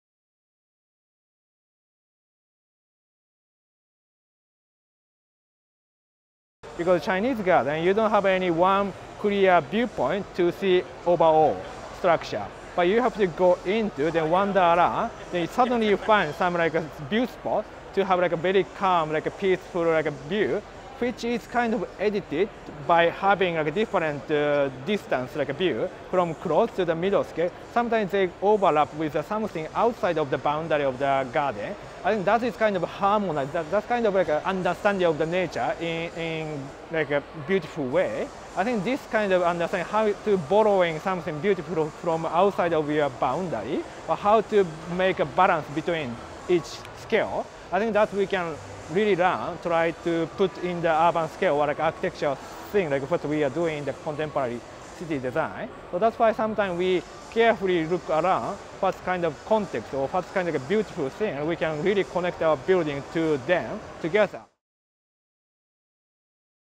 Yosuke Hayano - How does the 'Chinese Garden’ serve as inspiration for the modern city?
Q&A with Yosuke Hayano (Part 2 of 4)

Yosuke Hayano, MAD Architects, China and USA
2019 March Keynote Speaker

At Design Experience Series in March 2019, Yosuke Hayano, Principal partner at MAD, explored the idea of architecture as an emotional connection to nature. Hayano shared his views on the future of cities and the importance of spaces for people to reflect and spiritually connect with their surrounding environment, and the challenge of our generation to find a methodology that harmonises architecture with diverse natural environments.